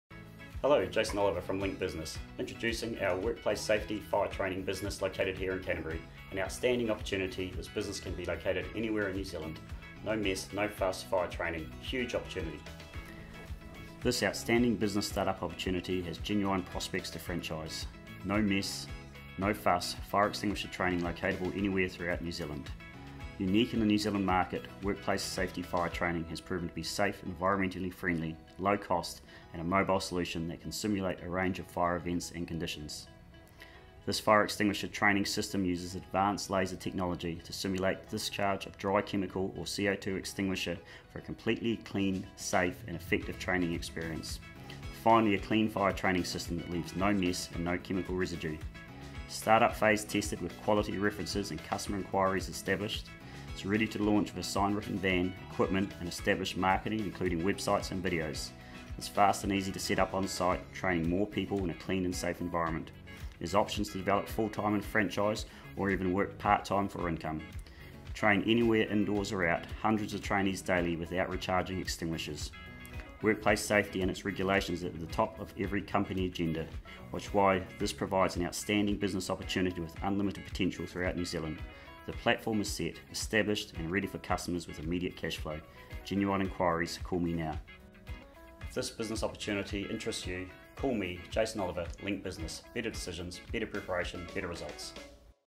No Fuss – No Mess Fire Extinguisher training locatable anywhere throughout New Zealand.
Outstanding business start-up opportunity with genuine prospects to franchise.
Unique in the New Zealand market ‘Workplace Safety Fire Training’ has proven to be a safe, environmentally friendly, low-cost and mobile solution that can simulate a range of fire events and conditions.